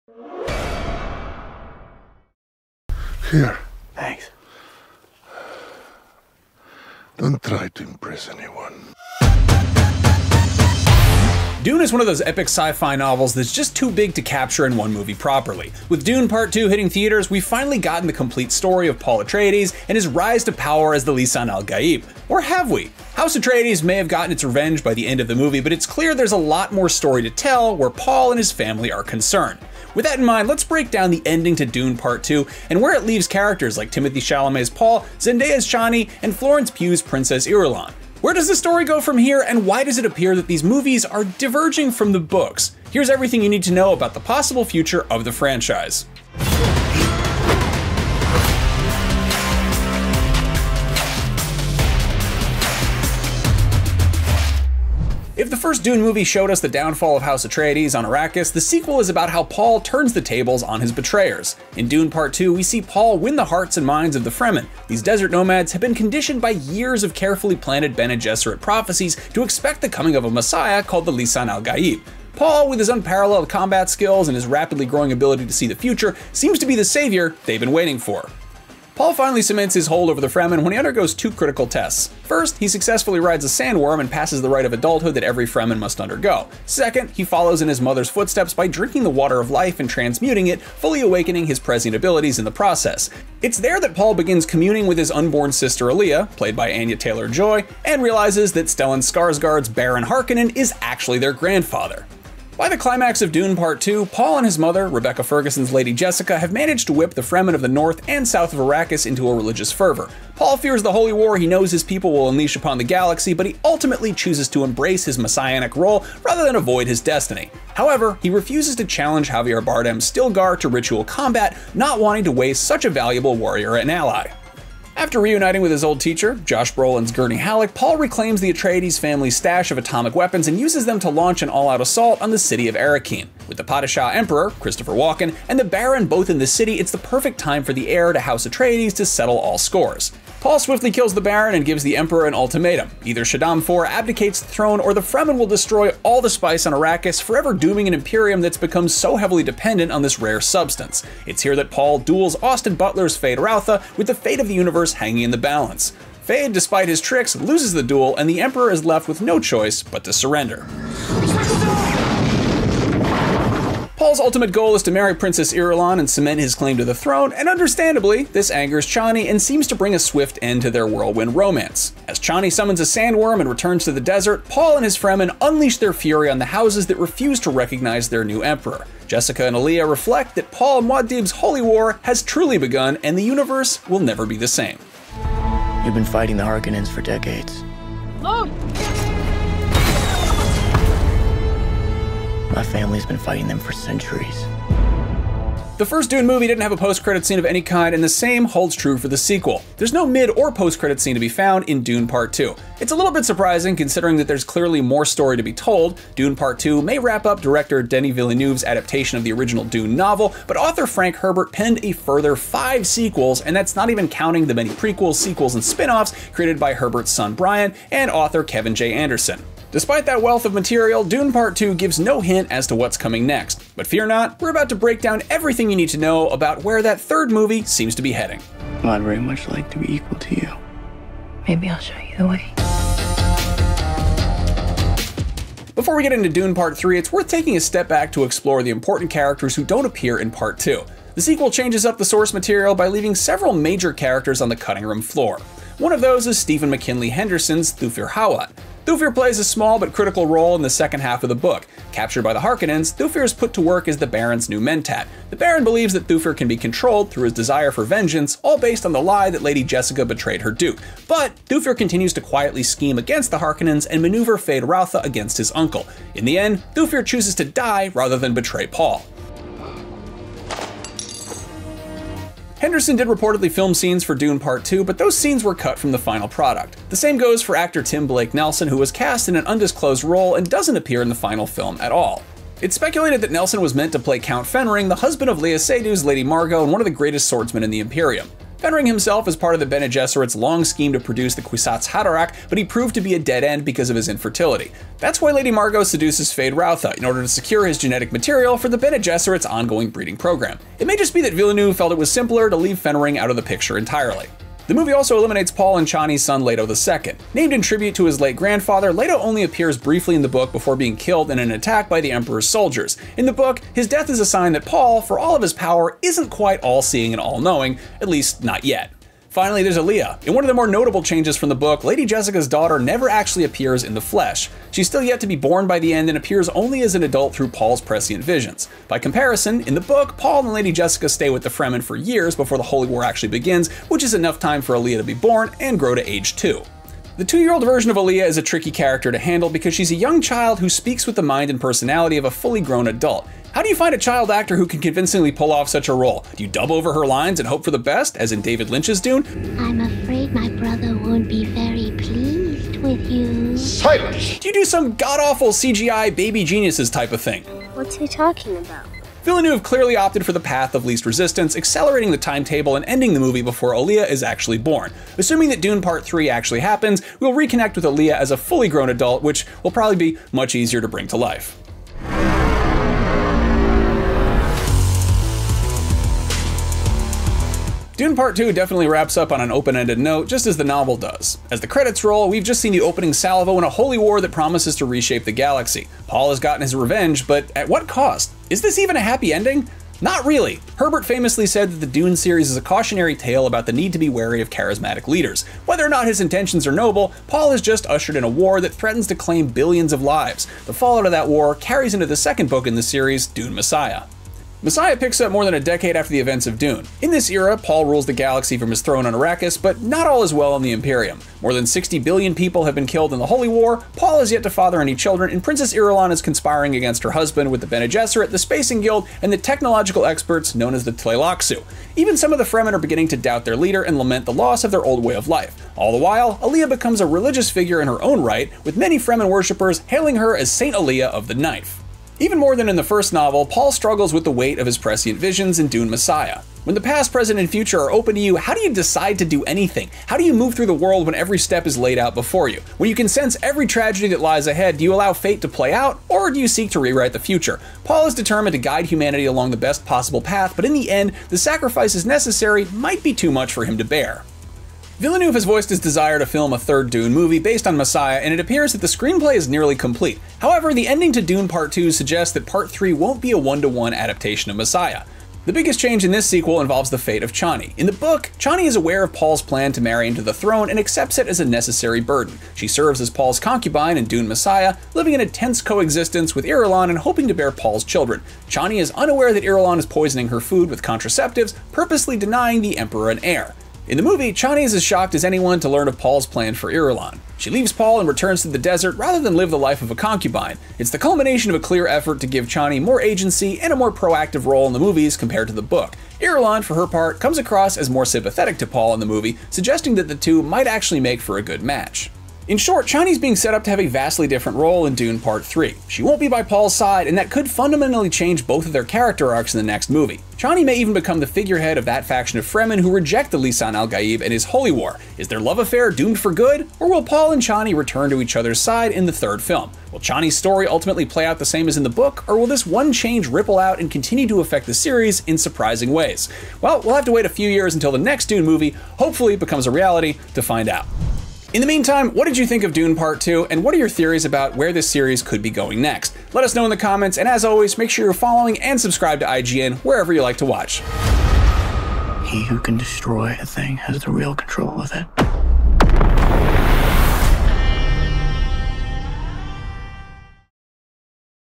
Dune: Part 2 Ending Explained - How the Sequel Sets Up Dune Messiah
Dune is one of those epic sci-fi novels that’s just too big to capture in one movie properly. With Dune: Part 2 hitting theaters, we’ve finally gotten the complete story of Paul Atreides and his rise to power as the Lisan al Gaib. Or have we? House Atreides may have gotten its revenge by the end of the movie, but it’s clear there’s a lot more story to tell where Paul and his family are concerned.

With that in mind, let’s break down the ending to Dune: Part 2 and where it leaves characters like Timothée Chalamet’s Paul, Zendaya’s Chani, and Florence Pugh’s Princess Irulan. Where does the story go from here, and why does it appear that these movies are diverging from the books? Here’s everything you need to know about the possible future of the franchise.

Dune: Part 2 also stars Rebecca Ferguson, Josh Brolin, Stellan Skarsgård, Dave Bautista, Charlotte Rampling, Javier Bardem, Austin Butler, Christopher Walken and Léa Seydoux. The film is currently playing in theaters.